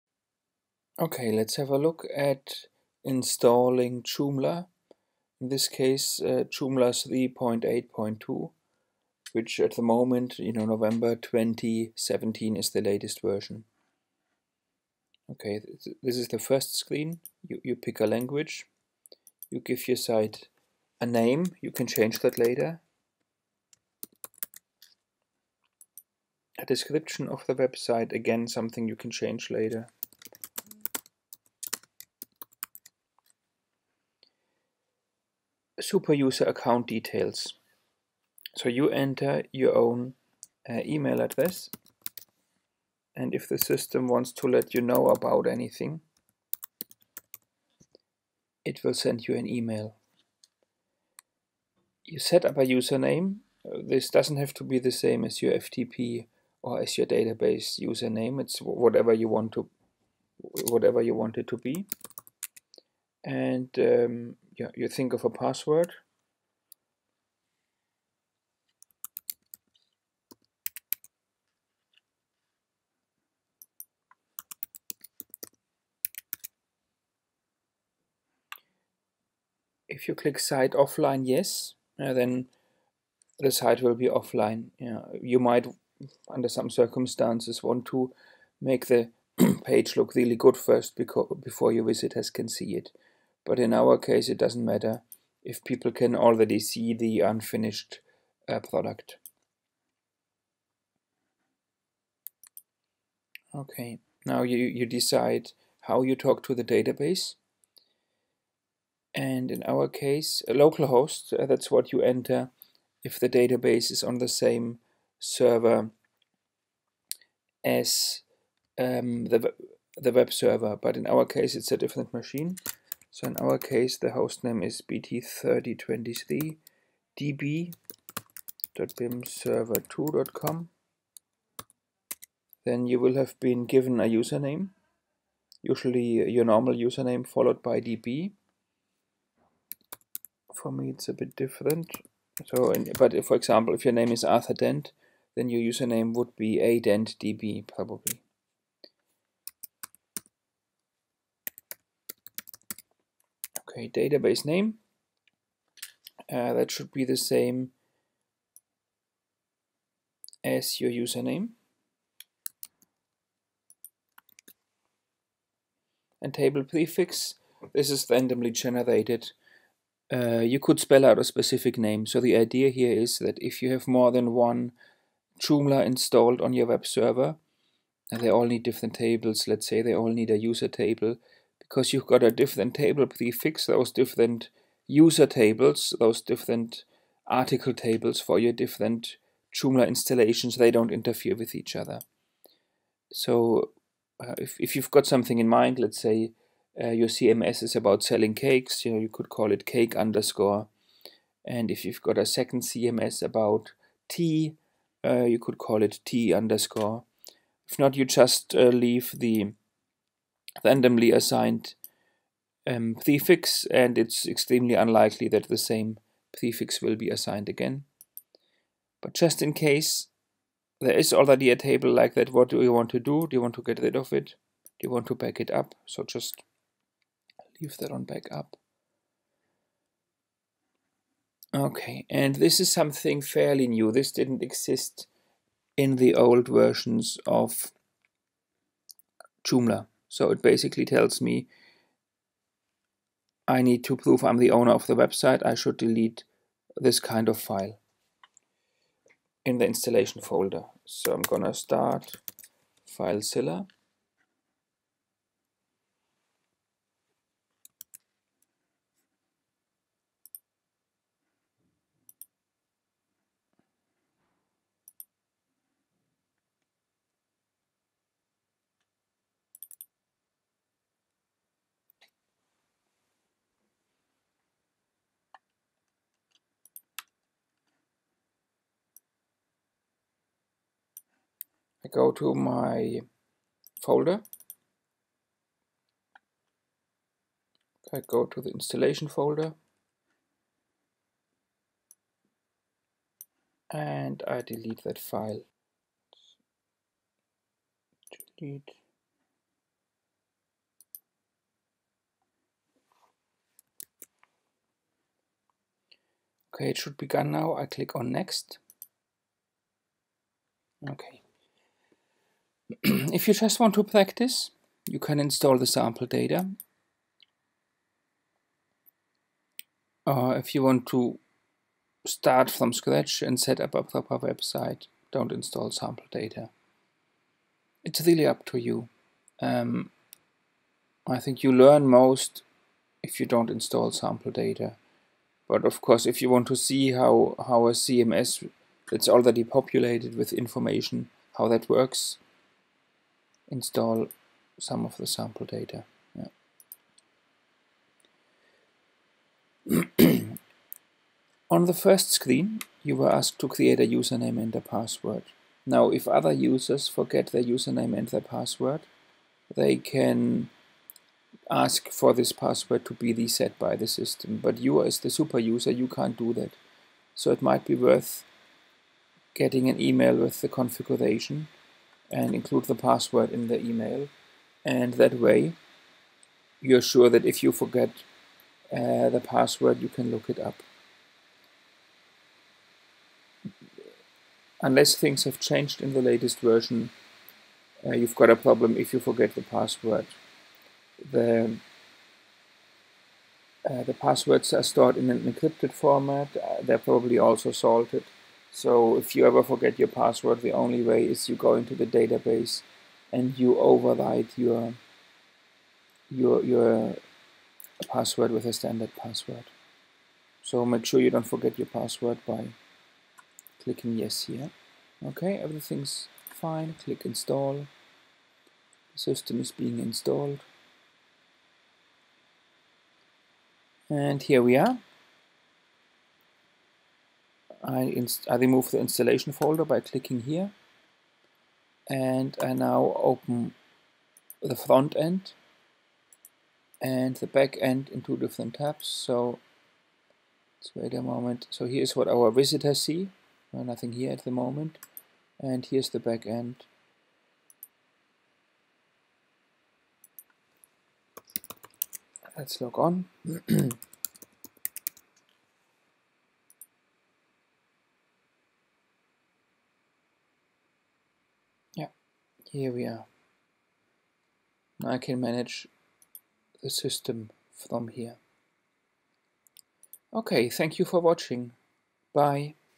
Joomla - Installing Joomla 3.8
A look at how to install Joomla 3.8.2 (The latest version in November 2017)